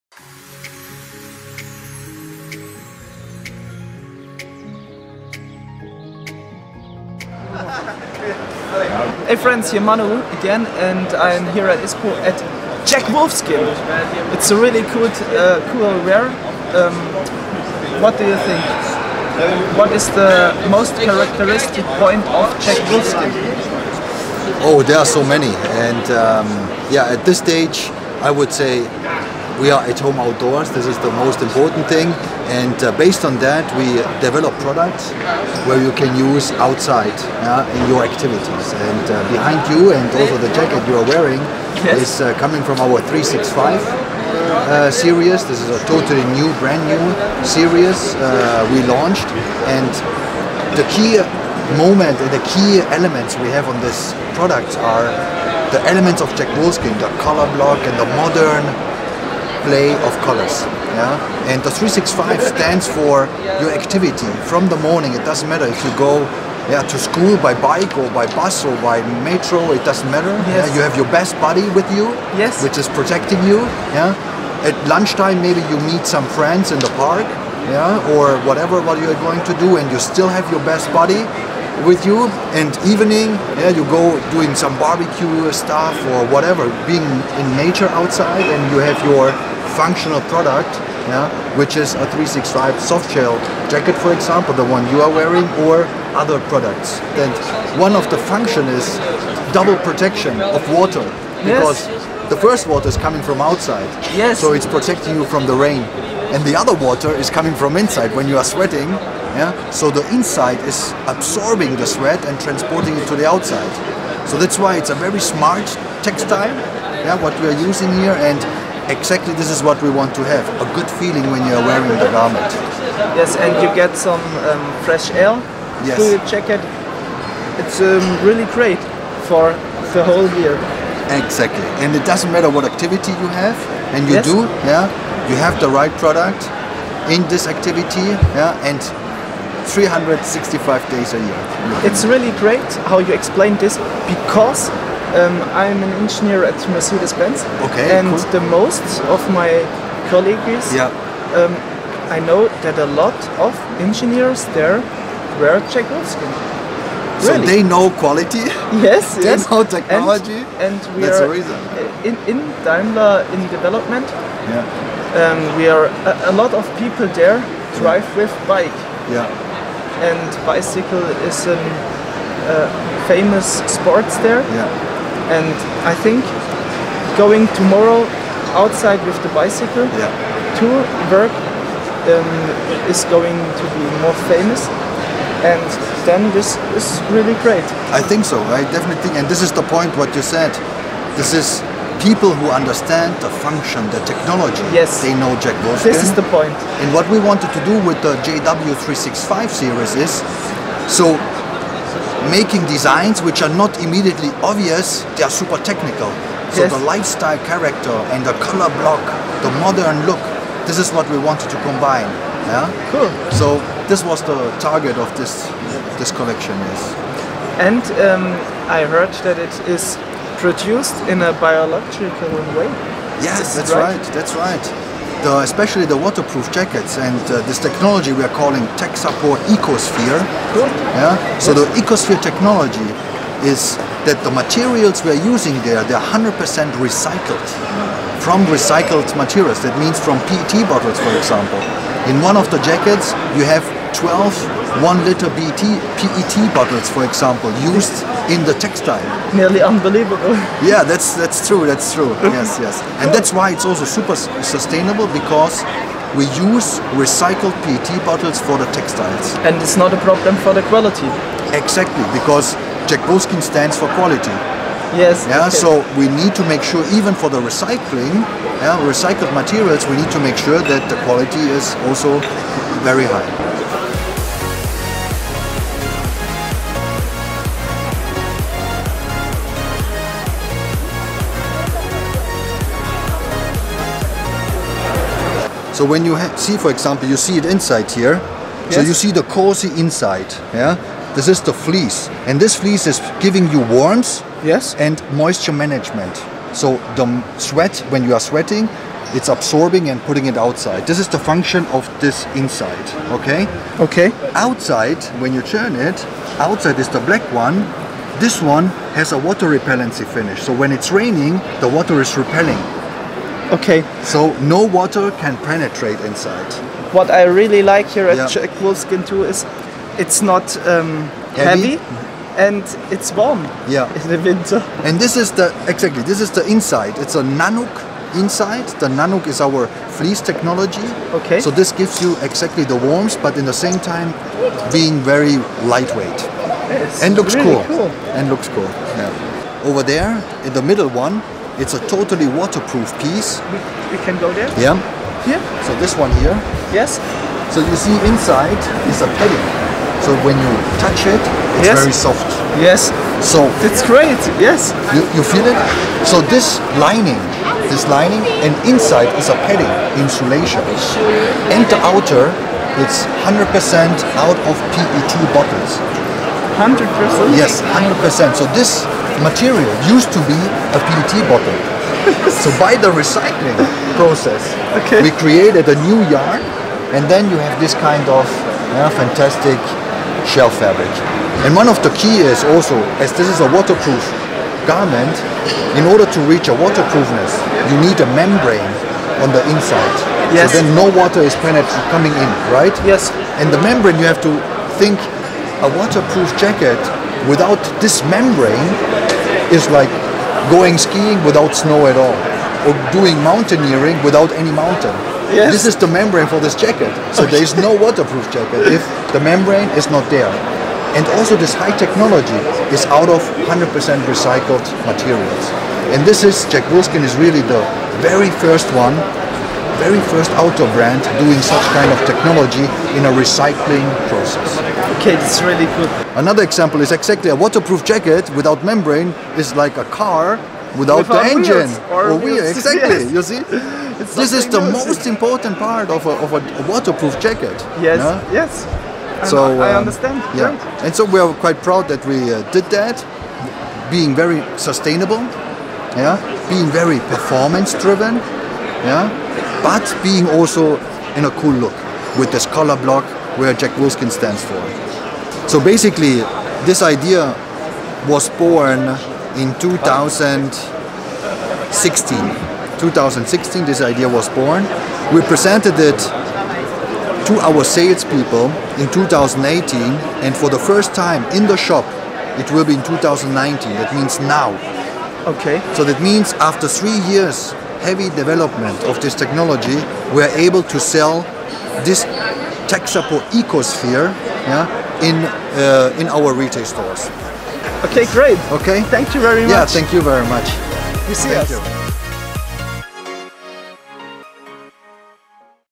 Manuel Teufel Nr 50: Interview with Daniele Grasso (Director Apparel) Jack Wolfskin / ISPO 2019
Daniele Grasso explains the new collection and the use of recycled materials (jackets made from old PET water bottles for example).